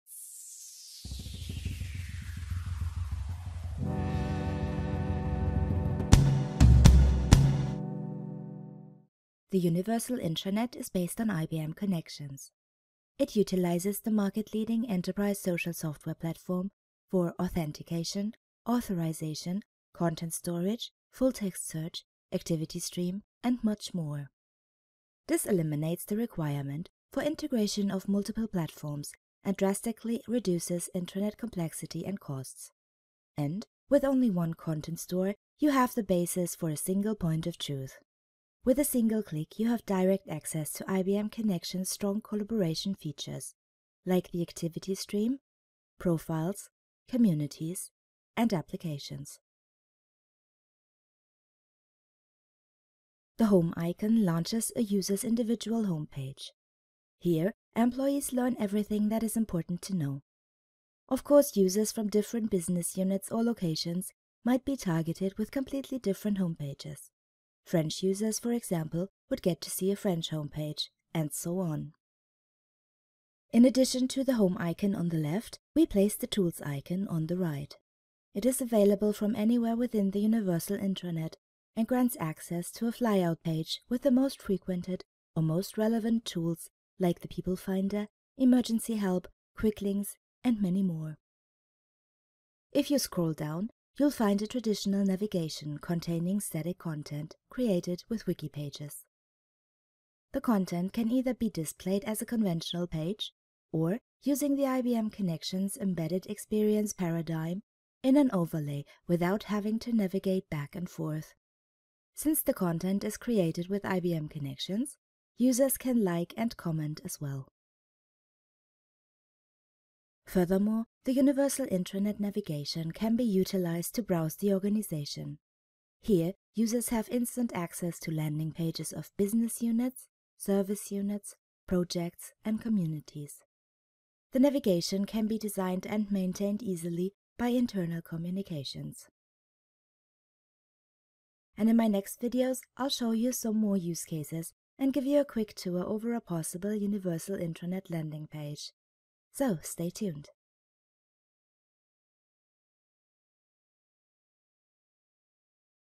Universal Intranet - A Truly Integrated User Interface
Because Integration Matters! Create a Universal Intranet based on IBM Connections,  IBM Connections Engagement Center (aka XCC - Web Content & Custom Apps Extension) and IBM Forms Experience Builder.